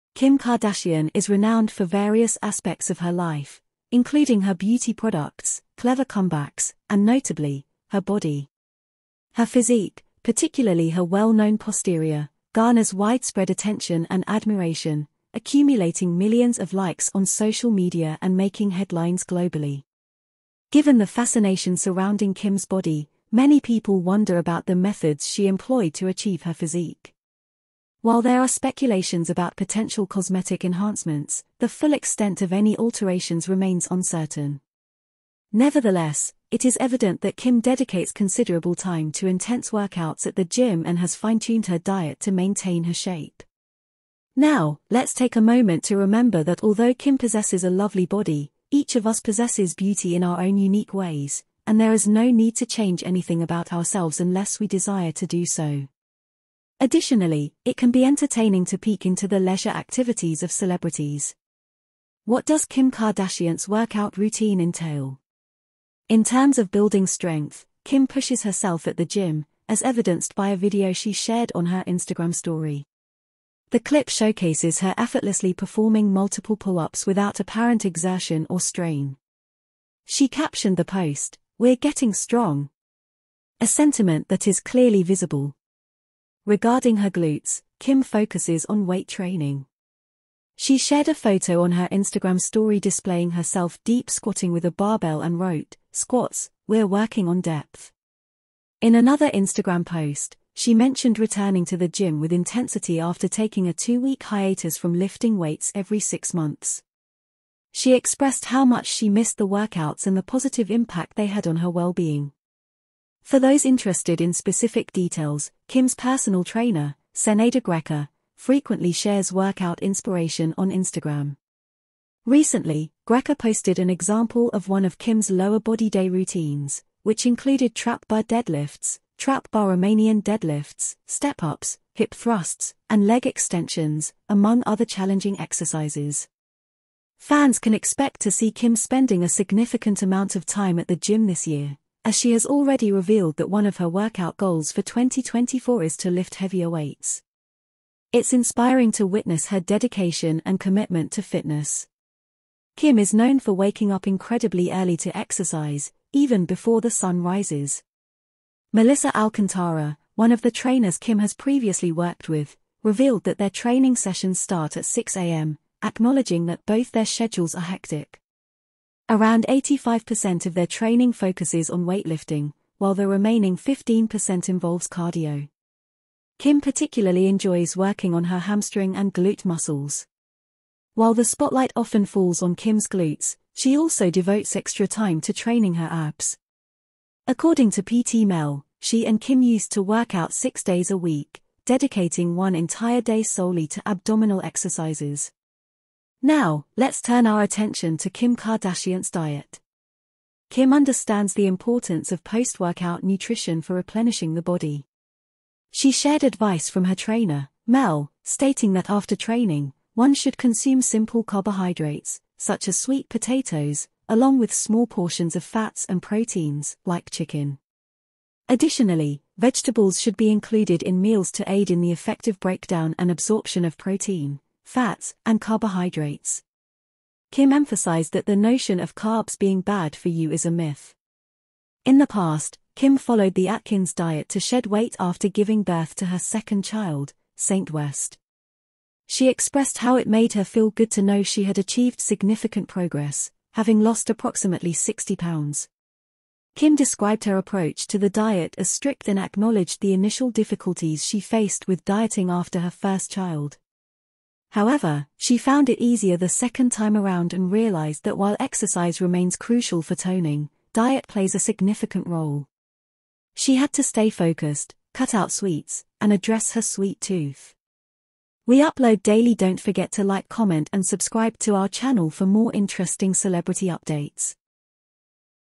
Kim Kardashian gym Workout: How she Keep FIT!
Kim Kardashian is renowned for various aspects of her life, including her beauty products, clever comebacks, and notably, her body. Her physique, particularly her well-known posterior, garners widespread attention and admiration, accumulating millions of likes on social media and making headlines globally.
Given the fascination surrounding Kim's body, many people wonder about the methods she employed to achieve her physique. While there are speculations about potential cosmetic enhancements, the full extent of any alterations remains uncertain. Nevertheless, it is evident that Kim dedicates considerable time to intense workouts at the gym and has fine-tuned her diet to maintain her shape.
Now, let's take a moment to remember that although Kim possesses a lovely body, each of us possesses beauty in our own unique ways, and there is no need to change anything about ourselves unless we desire to do so. Additionally, it can be entertaining to peek into the leisure activities of celebrities.
What does Kim Kardashian's workout routine entail? In terms of building strength, Kim pushes herself at the gym, as evidenced by a video she shared on her Instagram Story. The clip showcases her effortlessly performing multiple pull-ups without apparent exertion or strain. She captioned the post, "We're getting strong!"—a sentiment that is clearly visible.